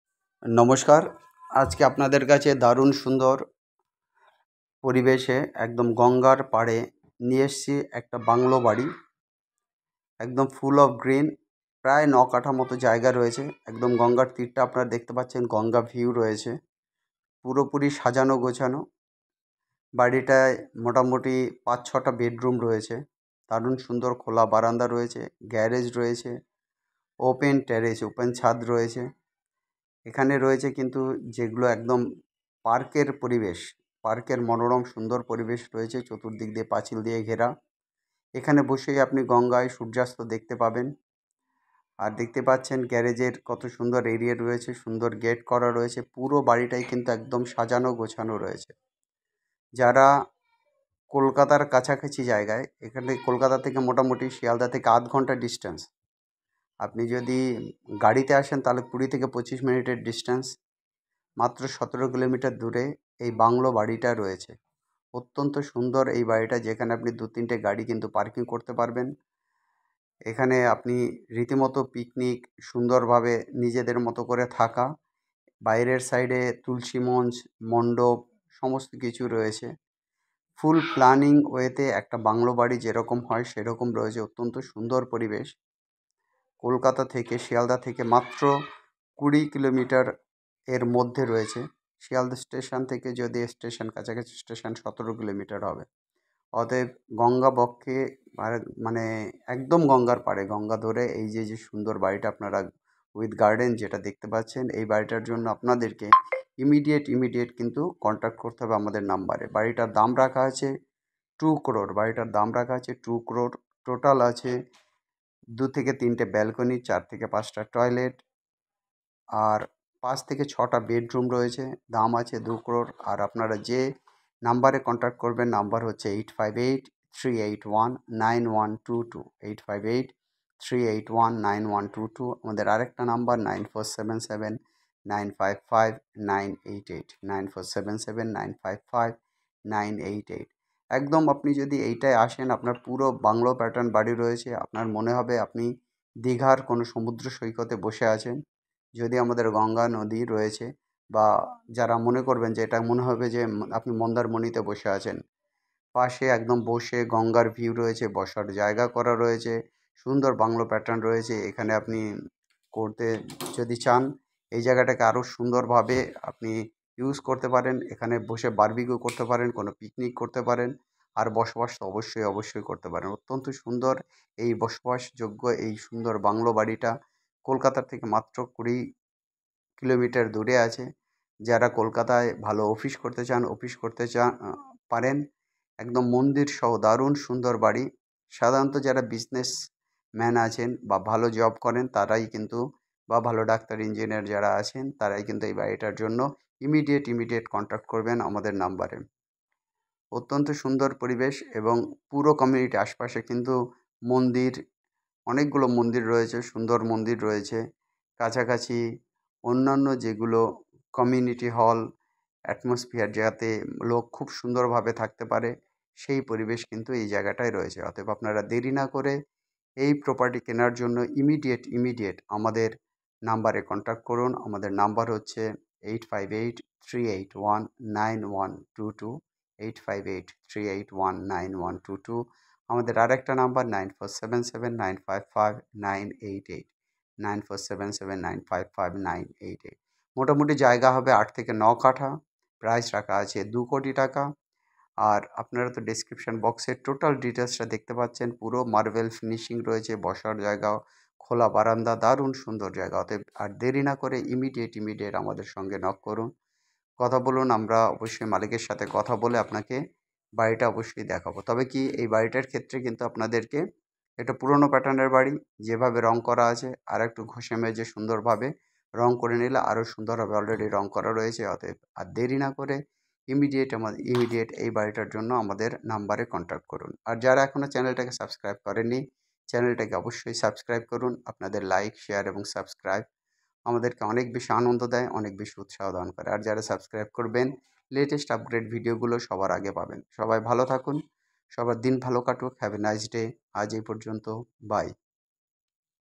6 bedroom bungalow house sale the Ganga riverside, 8-9 katha land, ₹2 Crore
Bungalow house sale beside the Ganga river- 17 km distance from Shialdaha railway station. Five to six bedrooms, 2-3 balconies, 4-5 toilets full of green and natural ambiences feel like paradise.

Approximately 9 katha land area. Nearest railway station khardah is 2 km distance from the house, BT road is 1.3 km distance.

Price ₹2 Crore.